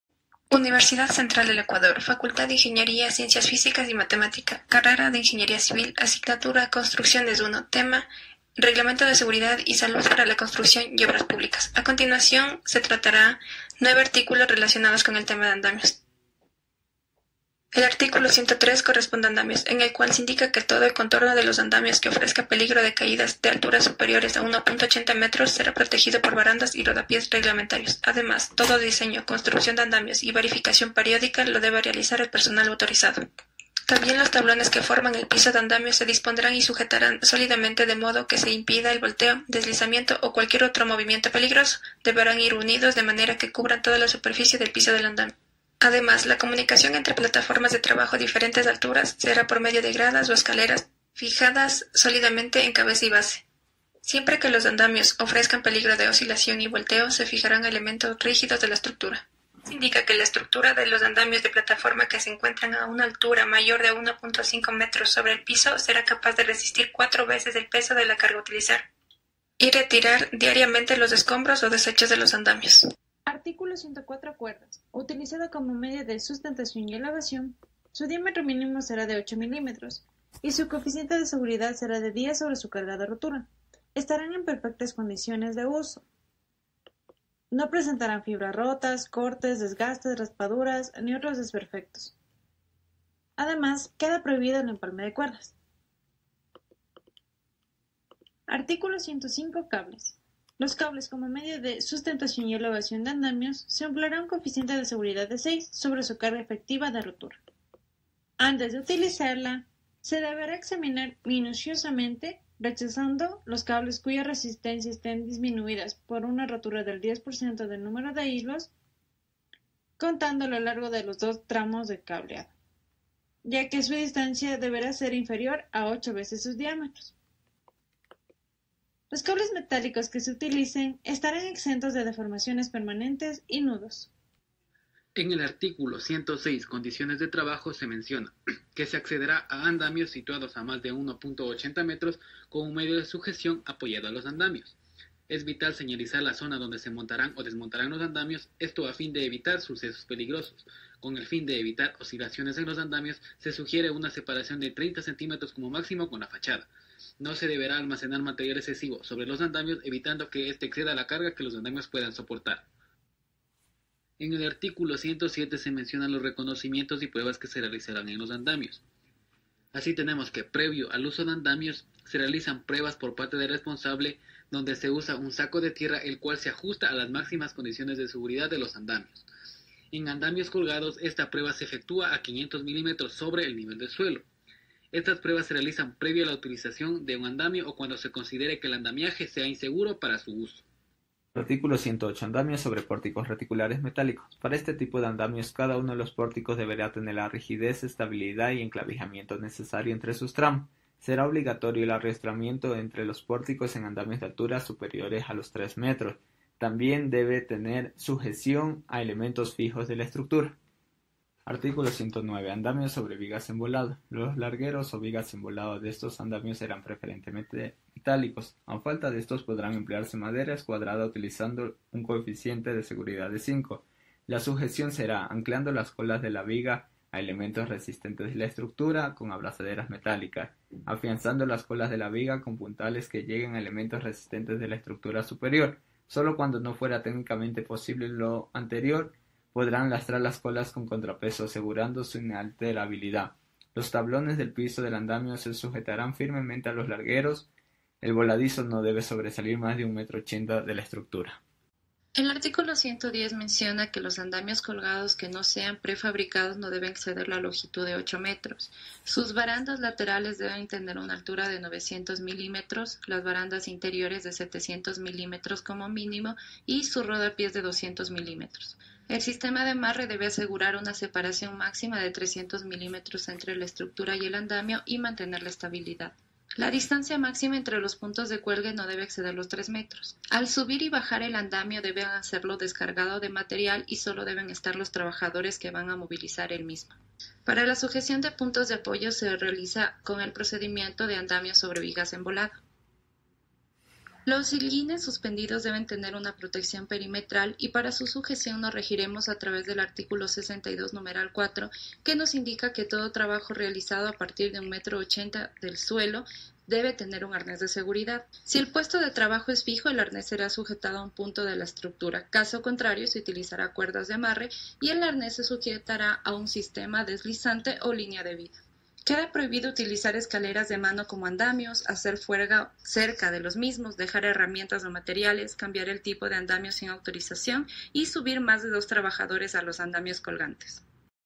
Uso de Andamios
El andamio es una estructura auxiliar o construcción provisional con la que se pueden realizar desde torres hasta pasarelas o puentes.
En el presente video se explica algunos artículos que corresponden a los andamios según el Reglamento de Seguridad y Salud para la Construcción y Obras Públicas del Ecuador.
Un aporte de: Alquinga Luis, Catucuago Miriam,  Delgado Katherine,
 García Patricia, Gómez Jean. Estudiantes de Ingeniería Civil